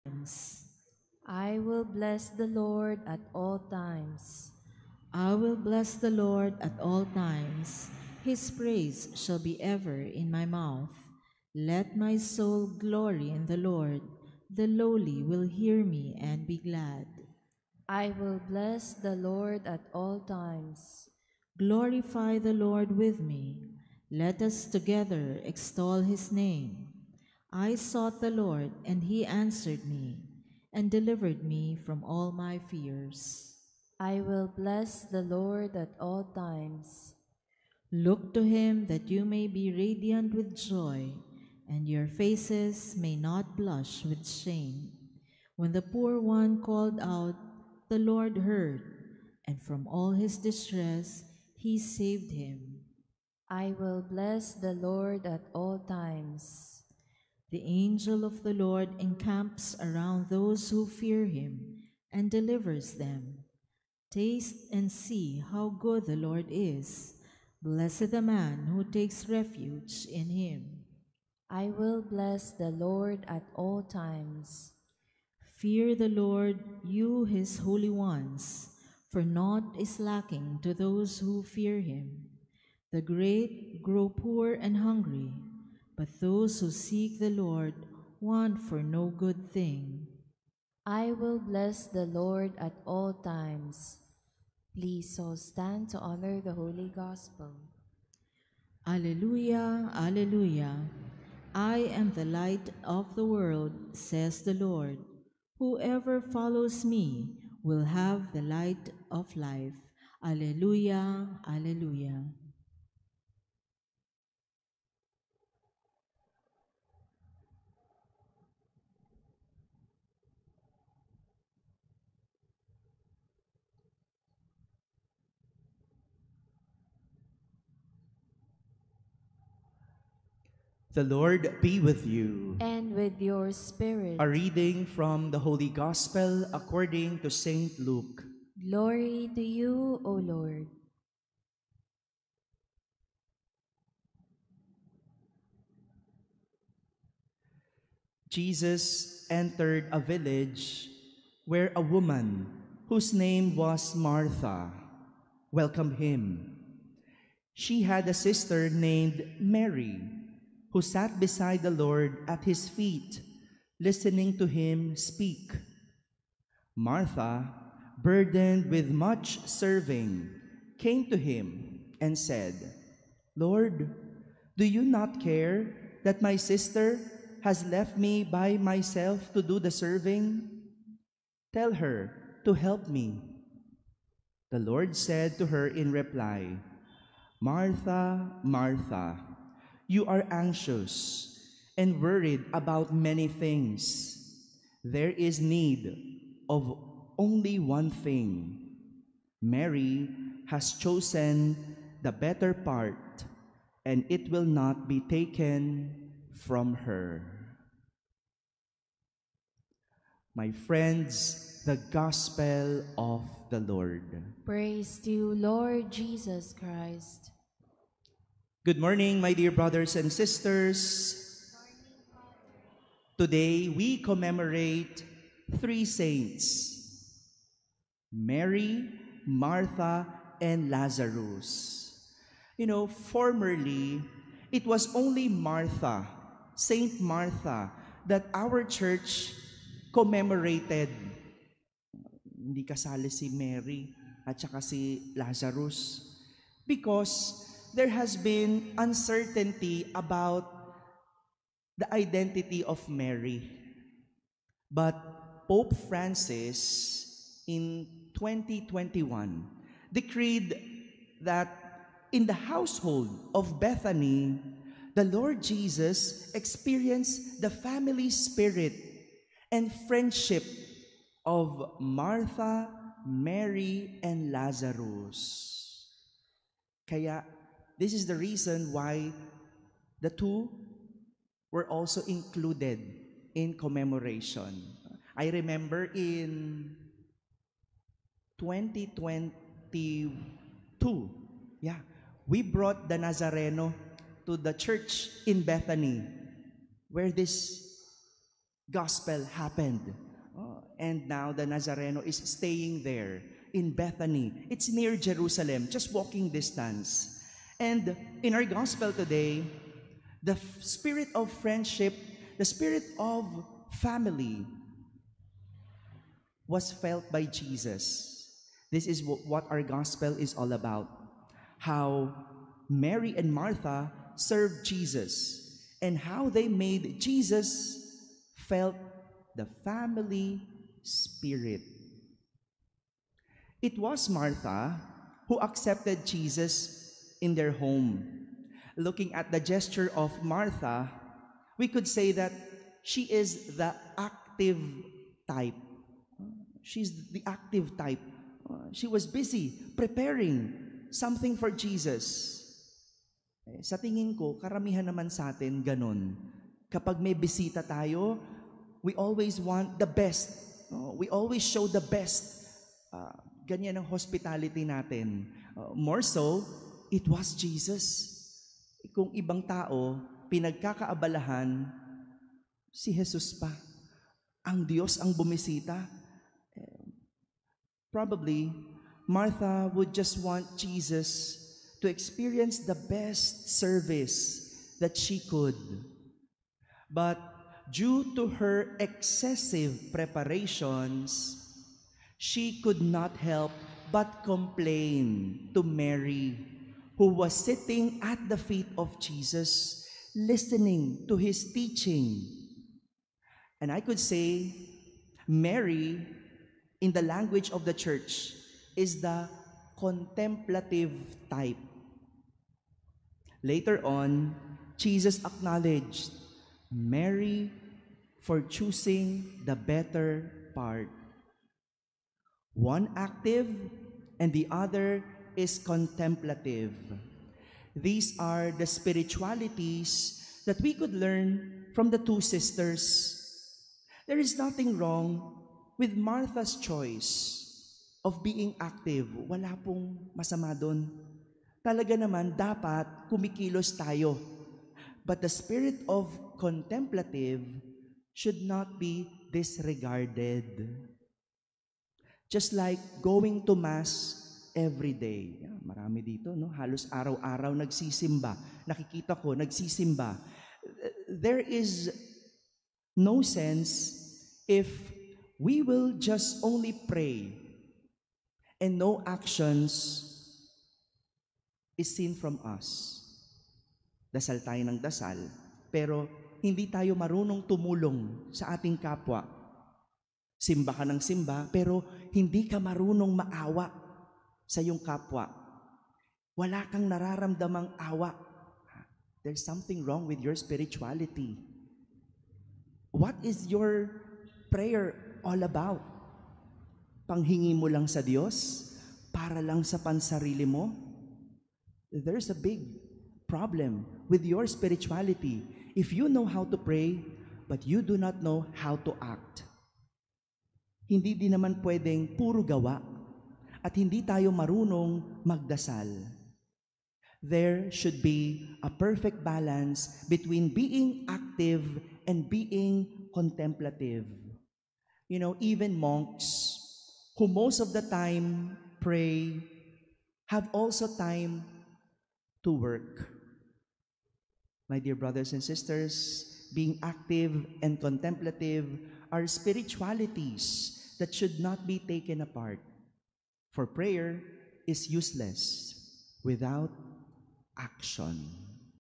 July 29, 2024 Rev. Fr. Danichi Hui Homily
Monday of the Seventeenth Week in Ordinary Time
Memorial of Saints Martha, Lazarus, and Maria
Archdiocesan Shrine of Nuestra Señora de Guia